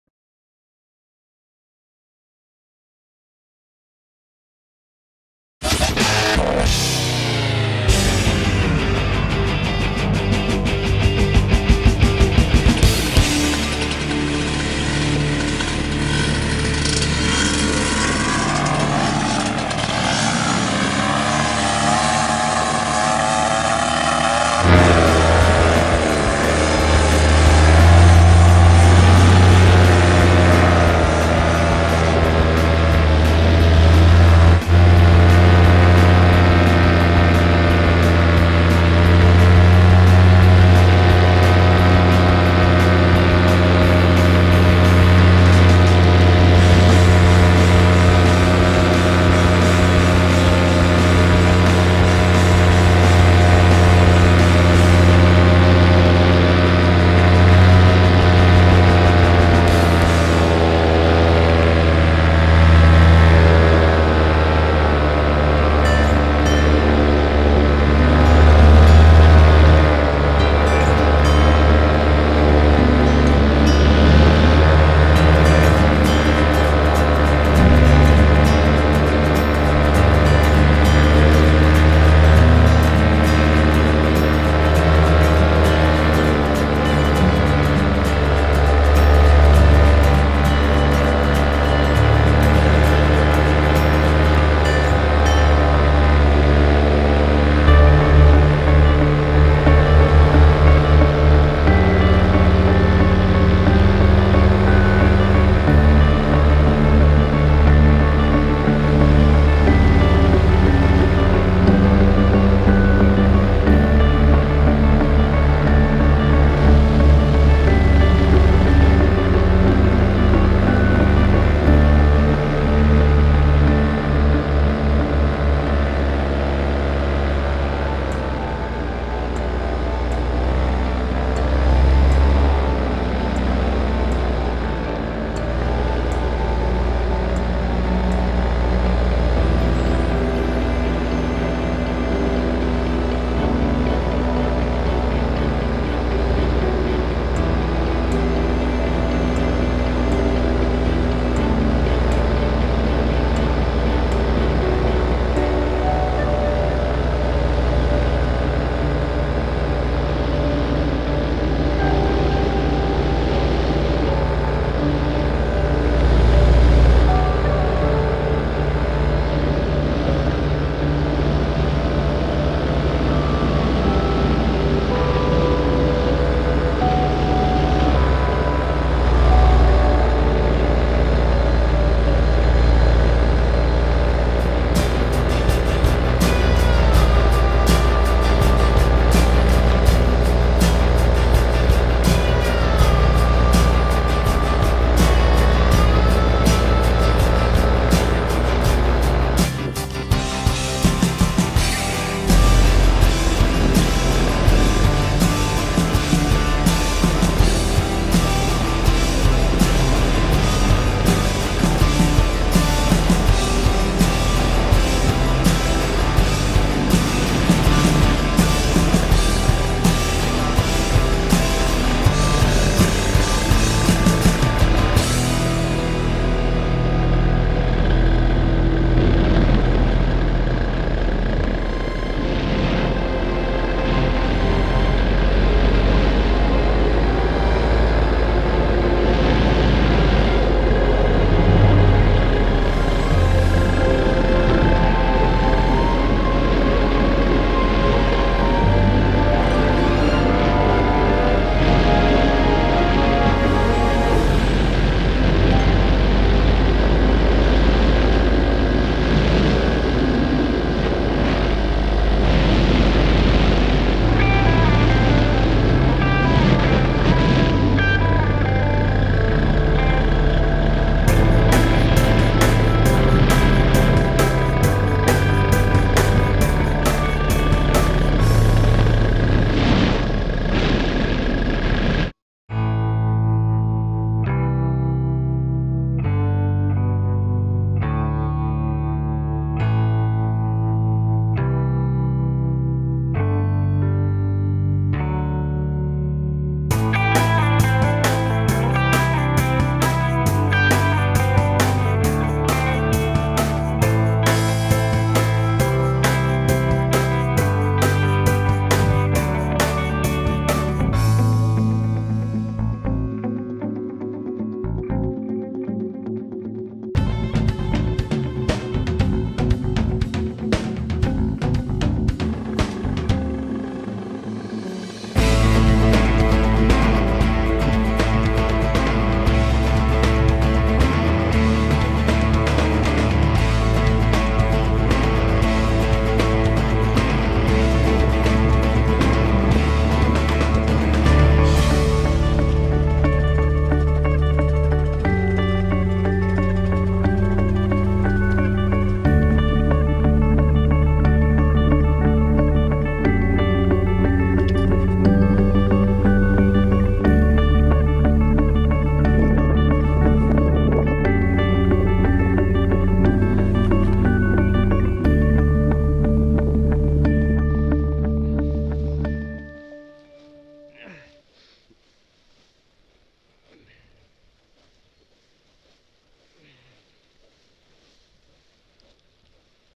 UKPPG - Stuarts 1st time Paramotoring in Scotland
First solo flight in Scotland after having being trained by UKPPG in March Cambridgeshire. Stuarts Flight was in quite thrmeic conditions and he handled it all very well.

What great scenary, cant wait to come and fly with you.

Well Done Stuart,

Nigel & Nikki
UKPPG